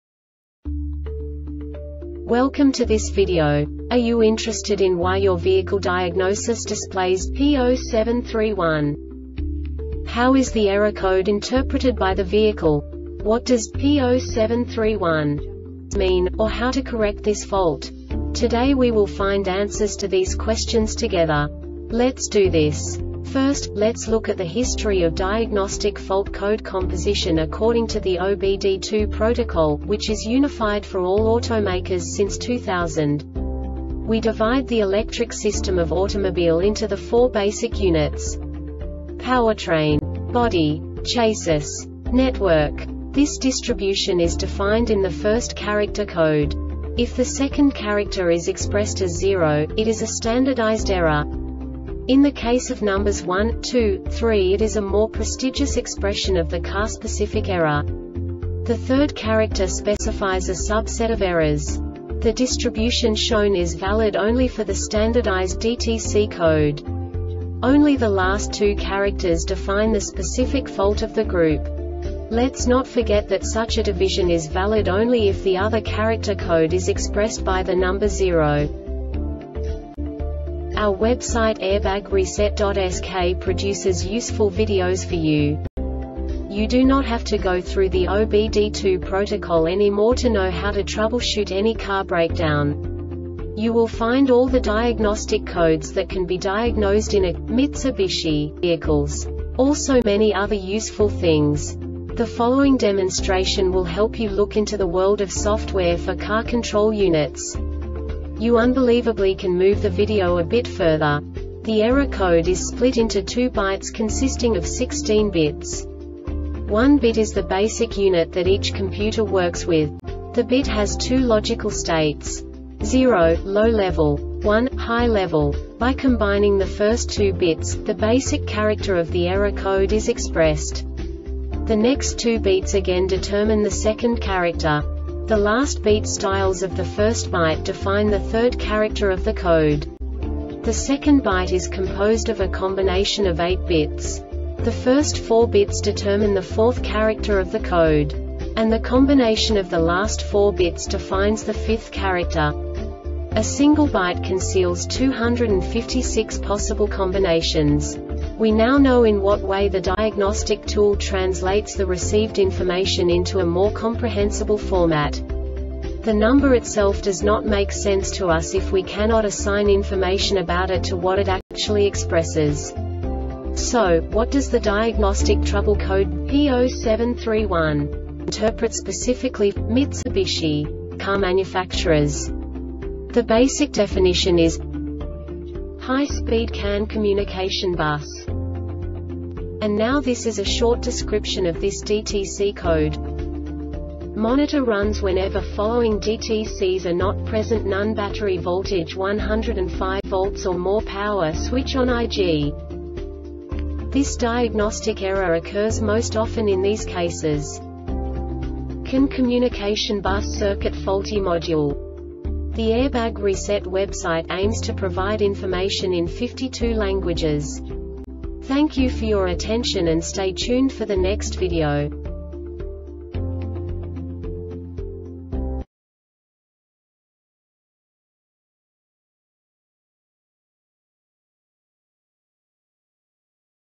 DTC Mitsubishi P0731 Short Explanation
Contents:
0:21 Basic DTC analysis according to OBD2 protocol standard.
1:48 Insight into programming
2:58 A diagnostically understandable description of the Diagnostic Trouble Code
3:05 Basic error definition

Make:
Mitsubishi
Code:
P0731
Definition:
High Speed CAN Communication Bus
Description: